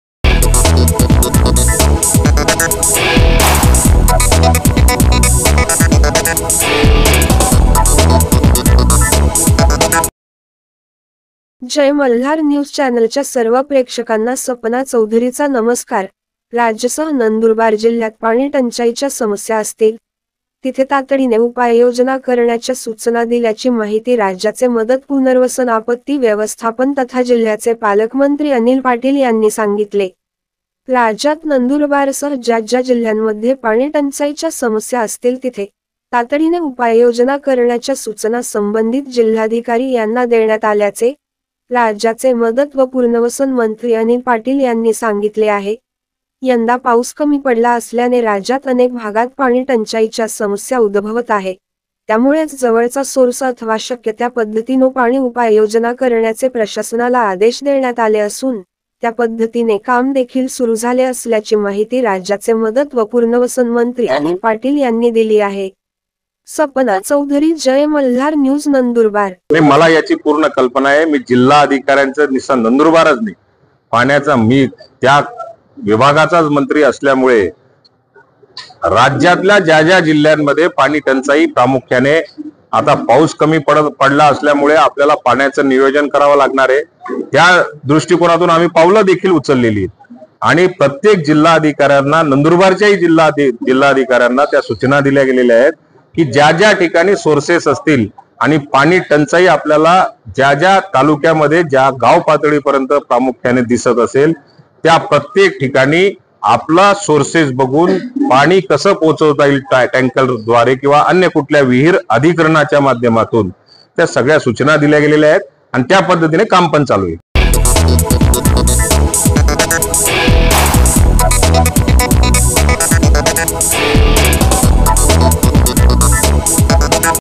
नंदूरबारजिल्ह्यात पाणीटंचाईच्या समस्याअसतील,तिथेतातडीने उपाययोजनाकरण्याच्या सूचना-मंत्री अनिल पाटील
राज्यसह नंदूरबार जिल्ह्यात पाणी टंचाईच्या समस्या असतील, तिथे तातडीने उपाययोजना करण्याच्या सूचना दिल्याची माहिती राज्याचे मदत पुनर्वसन आपत्ती व्यवस्थापन तथा जिल्ह्याचे पालकमंत्री अनिल पाटील यांनी सांगितले. राज्यात नंदुरबारसह ज्या ज्या जिल्हांमध्ये पाणी टंचाईच्या समस्या असतील तिथे, तातडीने उपाययोजना करण्याच्या सुचना संबंधीत जिल्हाधिकारी यांना देण्यात आल्याचे, राज्याचे मदत व पुर्नवसन मंत्री अनिल पाटील यांनी सांगितले आहे, यंदा पाऊस कमी पडला असल्याने राज्यात अनेक भागात पाणी टंचाईच्या समस्या उदभवत आहे, त्यामुळेच जवळचा सोर्स अथवा शक्यत्या पद्धतीनो पाणी उपाययोजना करण्याचे प्रशासनाला आदेश देण्यात आले असून, त्या पद्धतीने काम देखील सुरु झाले असल्याची माहीती राज्याचे मदत व पुर्नवसन मंत्री अनिल पाटील यांनी दिली आहे.